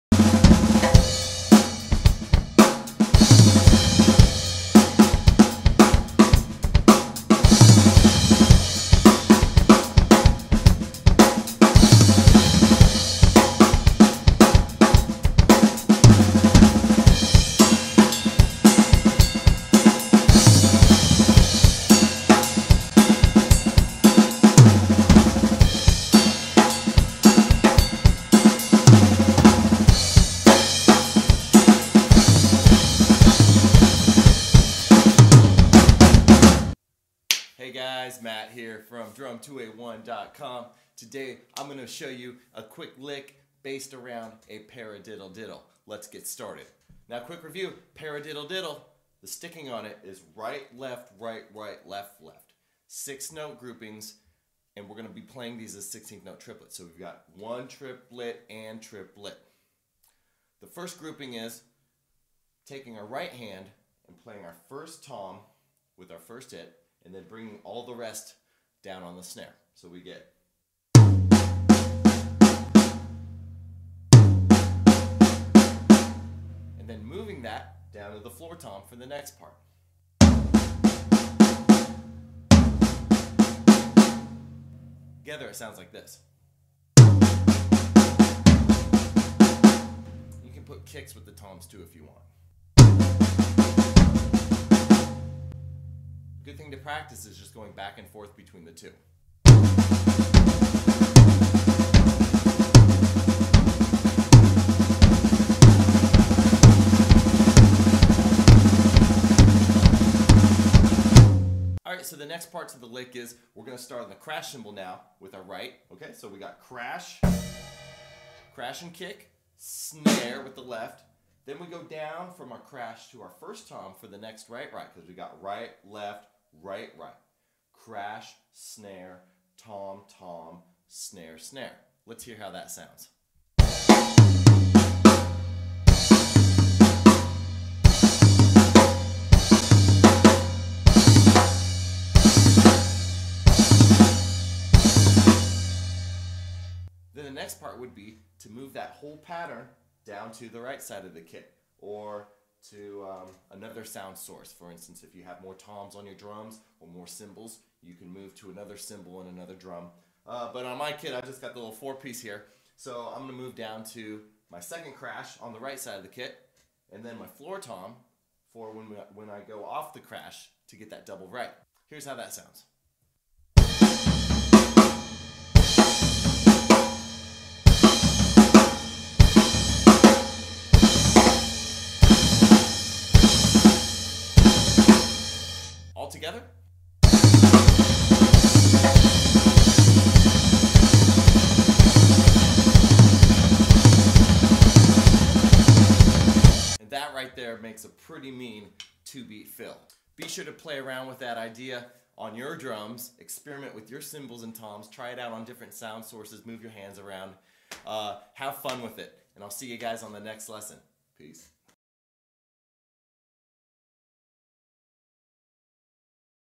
Paradiddle-diddle Fill - DRUM LESSON
Matt Smith shows you a cool lick based around a Paradiddle-diddle. Be sure to download the sheet music for this exercise at:

Have fun and keep practicing!